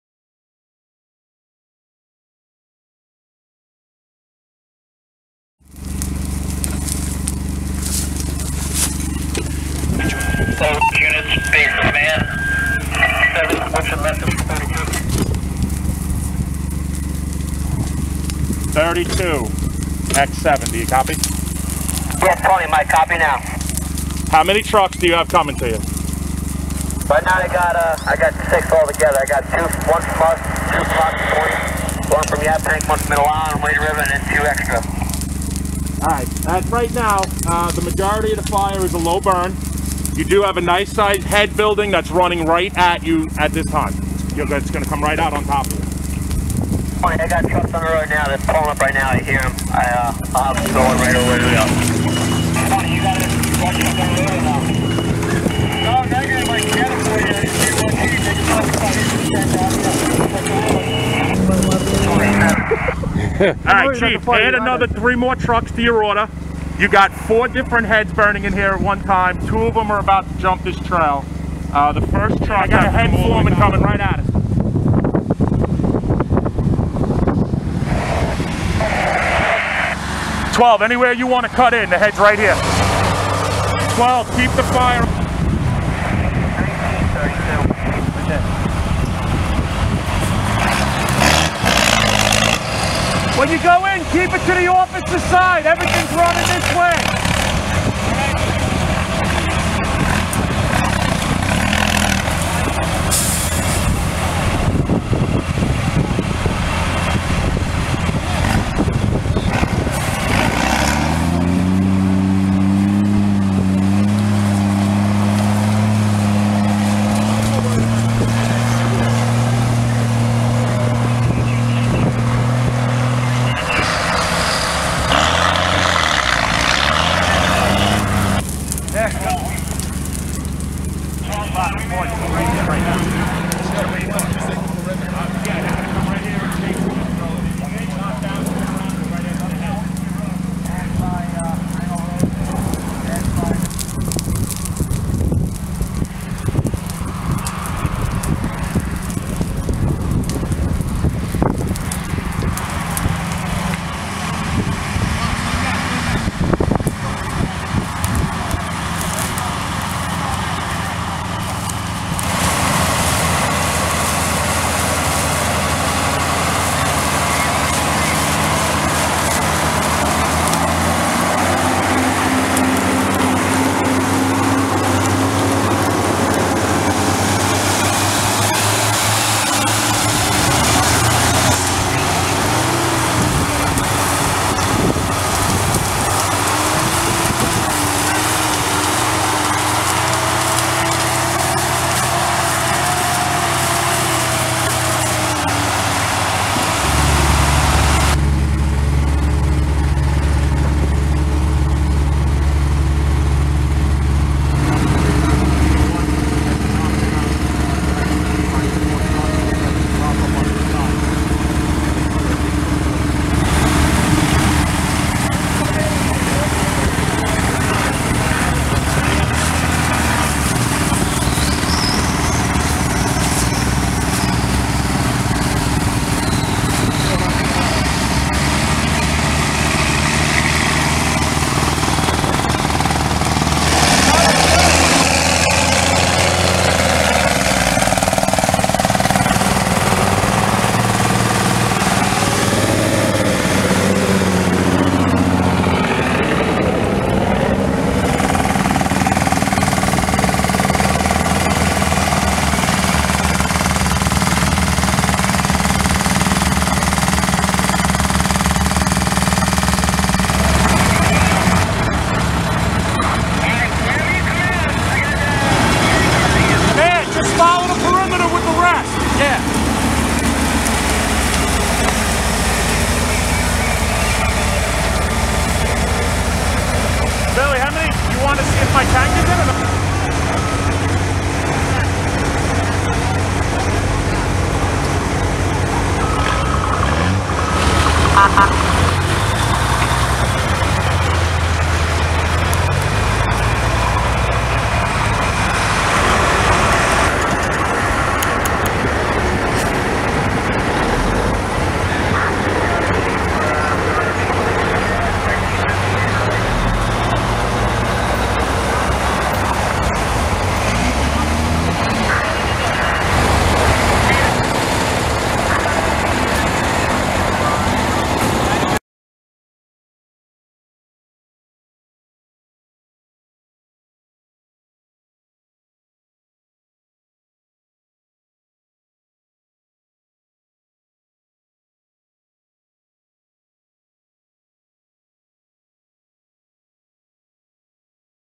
05 15 2014 1200 hrs Video Ridge Brush Lambui
Ridge Fire Dept. had this brush fire on Thurs., 05/15/2014 at approx. 12:00 hrs. and requested at least nine additional departments to respond on mutual aid. It took approx. three hours to extinguish the large area of brush with mop-up operations continuing in to the late afternoon.